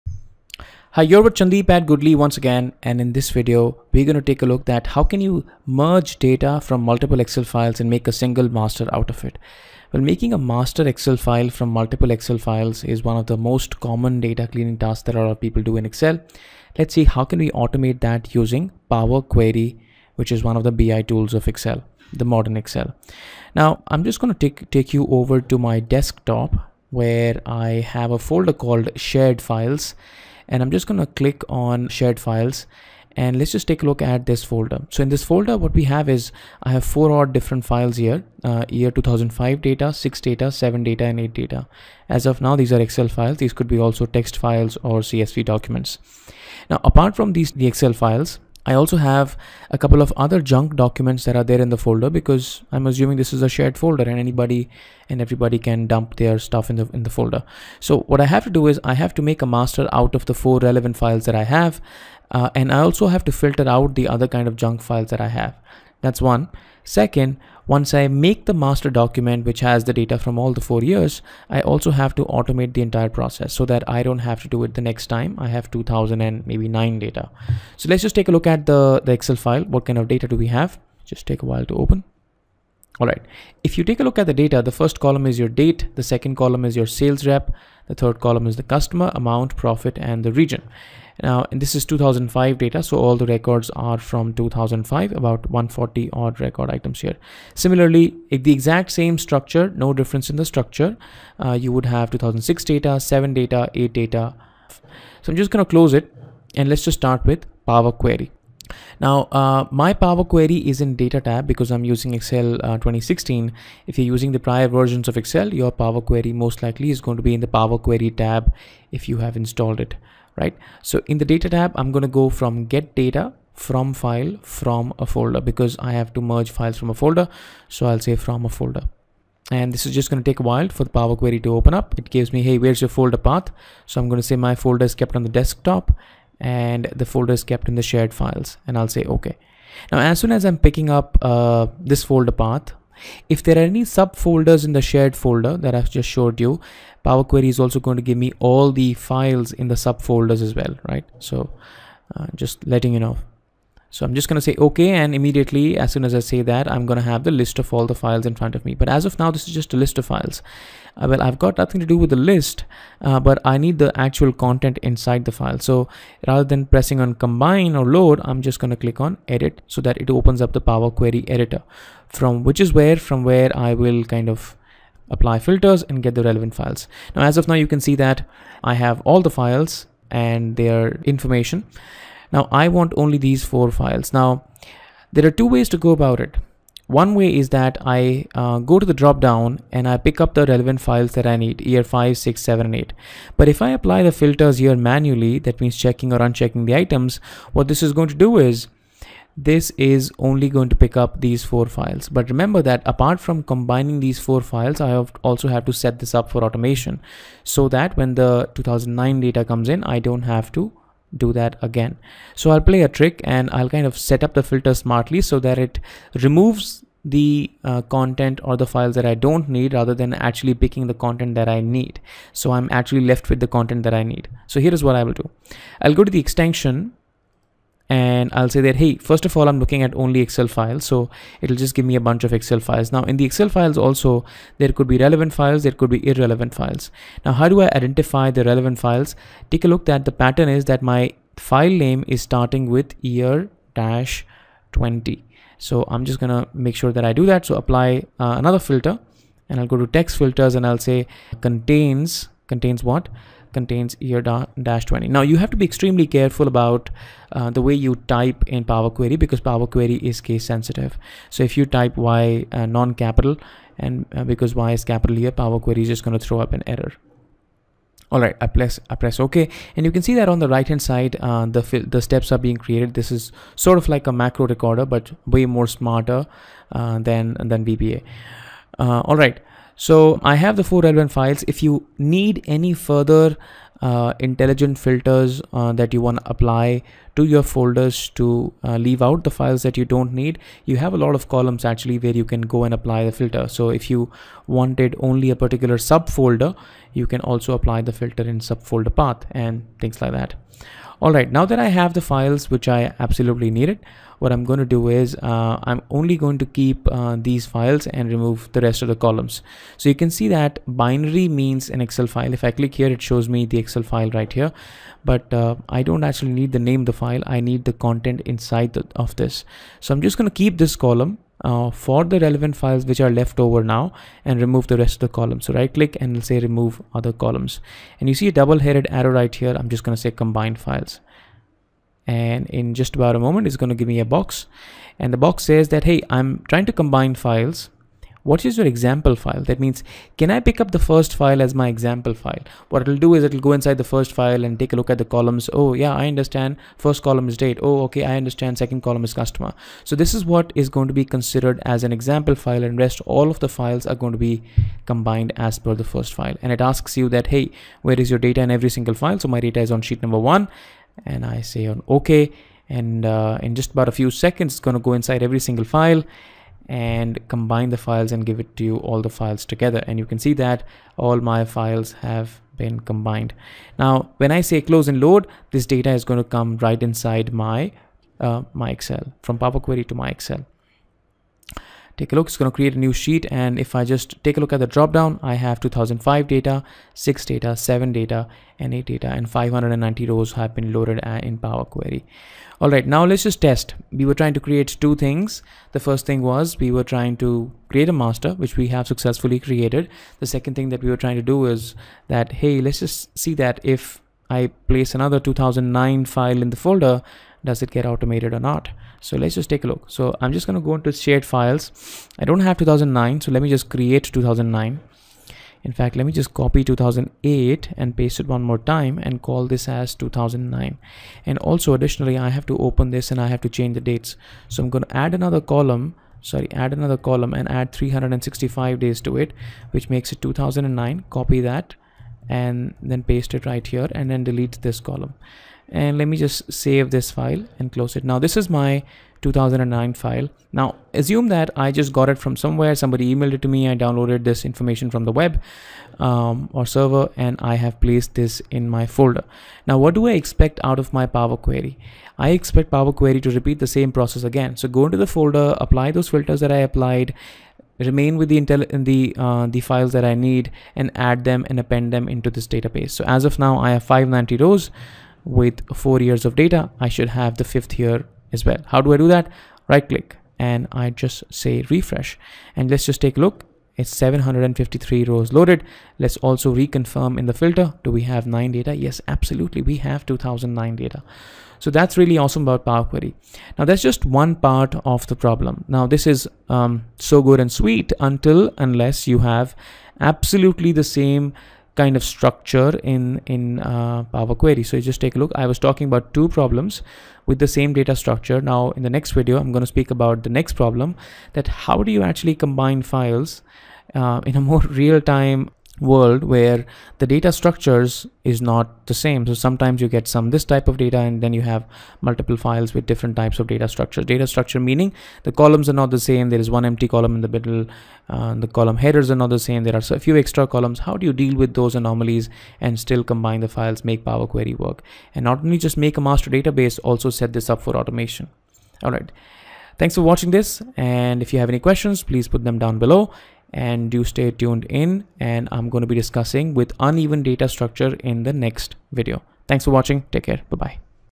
Combine Data from Multiple Excel Files in a Single Excel Sheet - Part 1
In this video you'll learn to combine data from multiple excel files into a single sheet using Power Query. In this case I am assuming that your data structure will be the same

- - - - My Courses - - - -
✔️ Mastering DAX in Power BI -